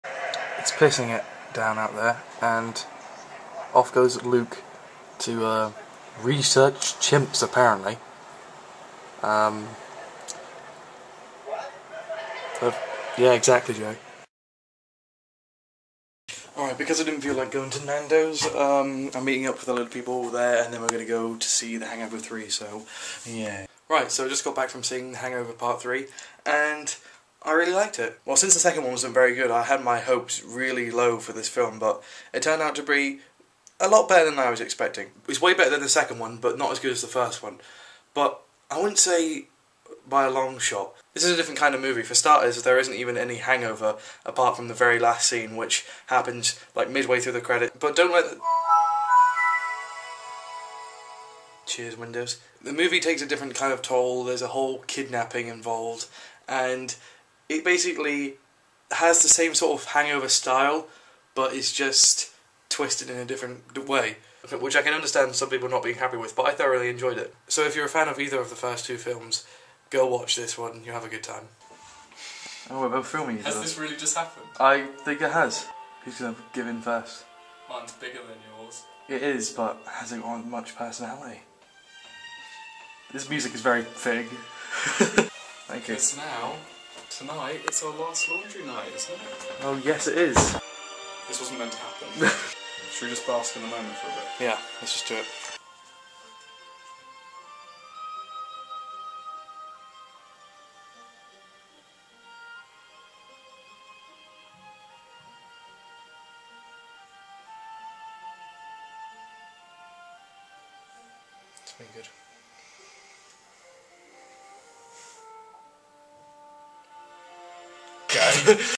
The End Of An Era
I went to see The Hangover Part III and give my opinions. But that's not the only Era that's ending... Luke and I share our last laundry night together :'(.